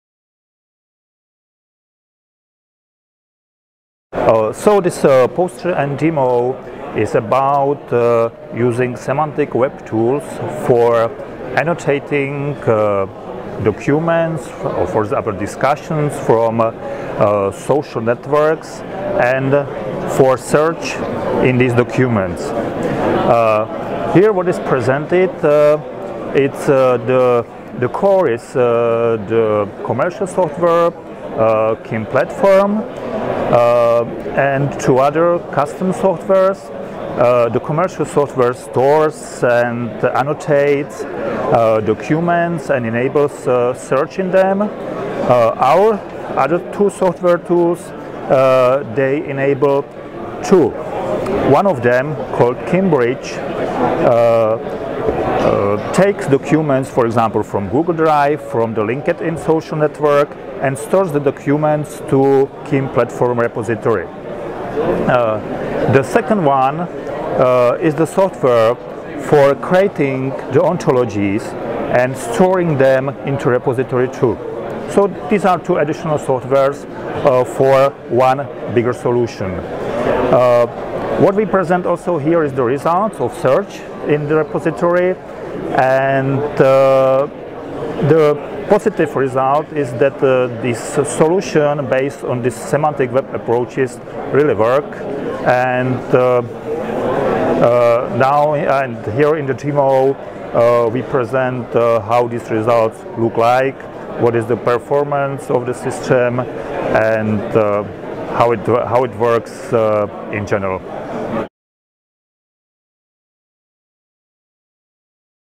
Roman Mouček presents tool for Electrophysiological Experiments
Computer demo at INCF Congress 2013, Stockholm, Sweden.
Presenter: Roman Mouček, University of West Bohemia, Plzen, Czech Republic

Construction and Use of Semantic Repository for Electrophysiological Experiments